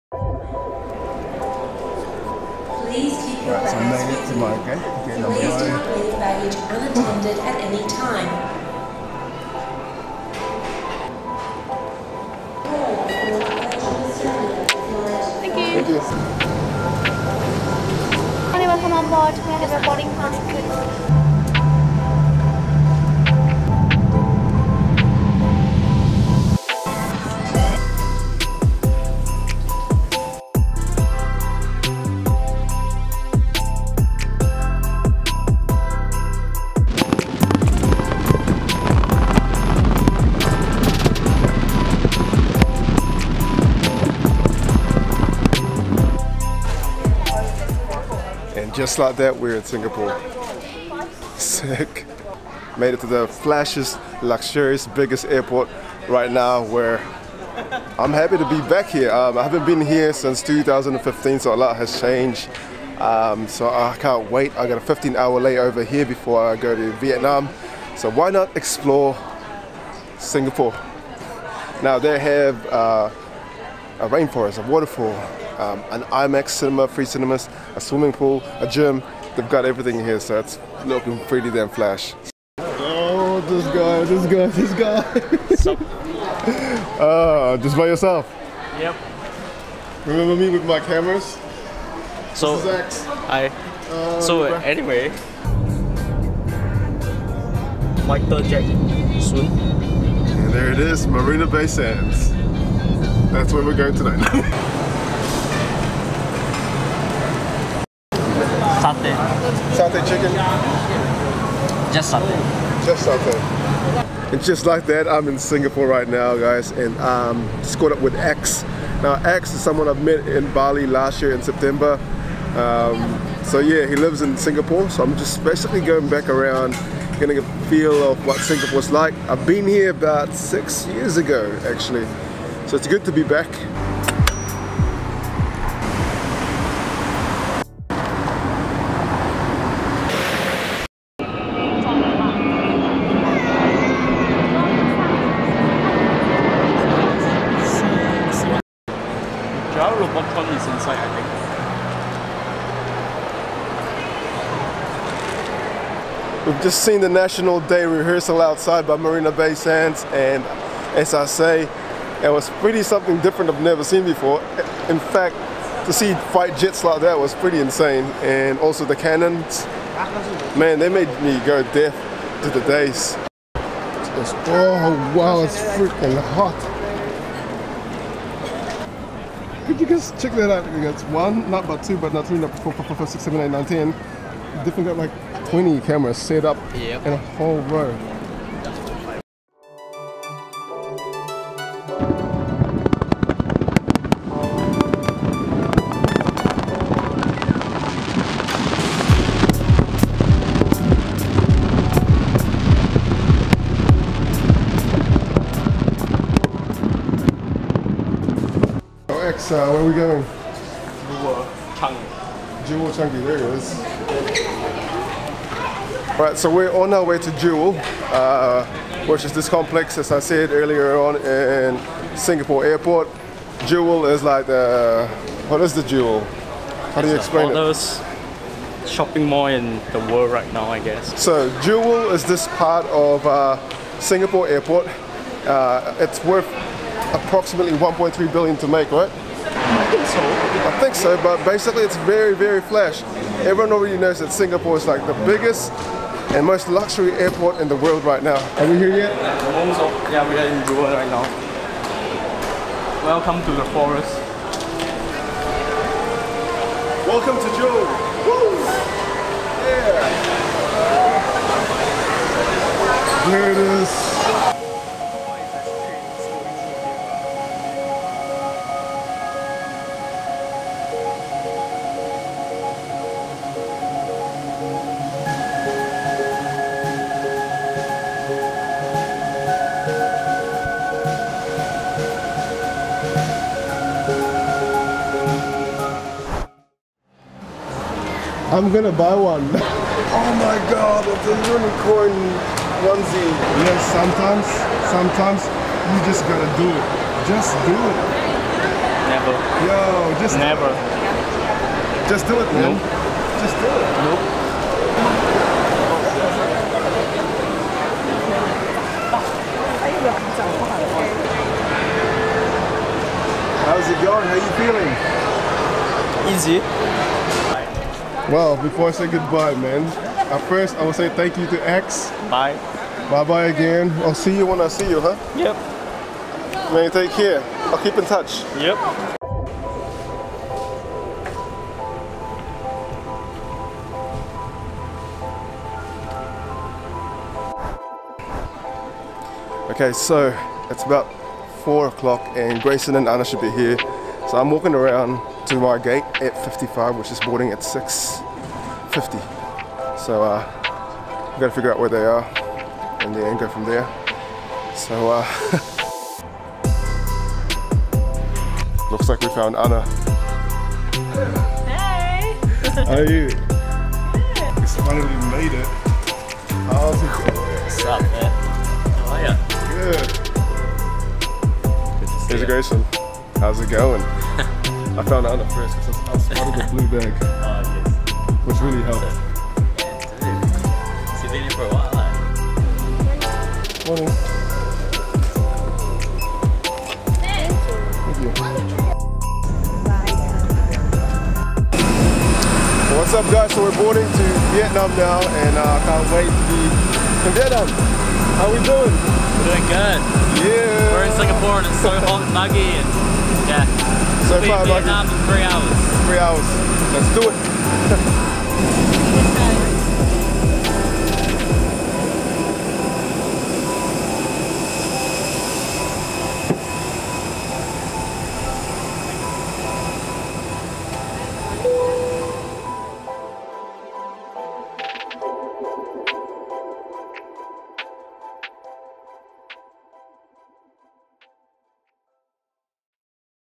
Singapore's Luxury Airport - National Day Parade (VLOG 50)
As you know I have recently taken a huge journey over the last 6 weeks that started from Australia. Which leads me to my main destination to VIETNAM.
But before heading to Vietnam
I had a stopover in Singapore to explore and see an old friend.

It's been about 6 years,  A lot has changed.
One of the places I wanted to see is the new complex
Changi Jewel which is the new extension to the airport.
Check out this vlog before you watch the Vietnam Vlog series which will come out soon!